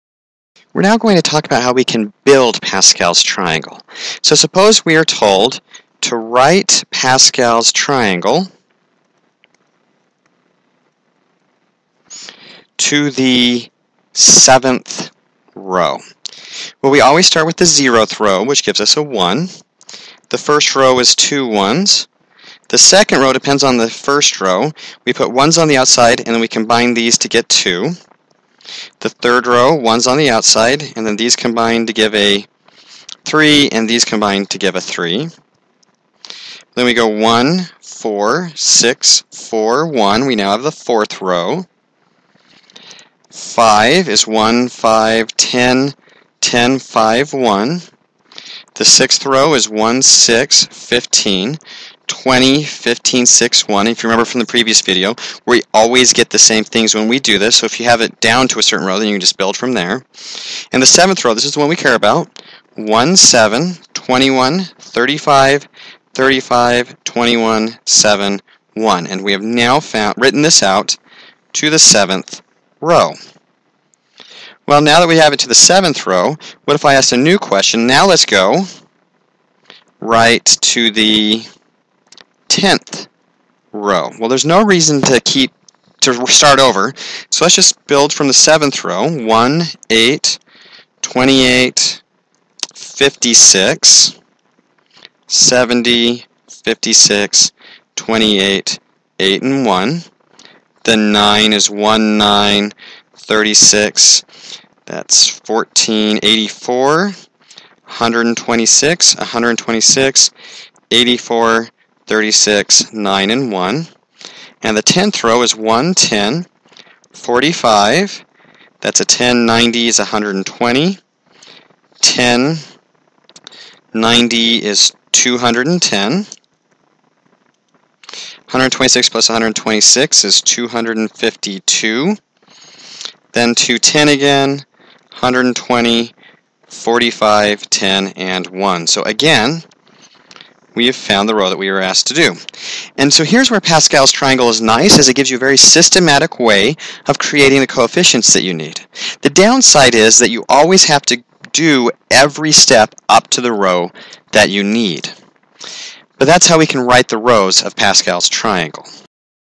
Write Pascal's Triangle to the nth row
This video shows how to find the nth row of Pascal's Triangle.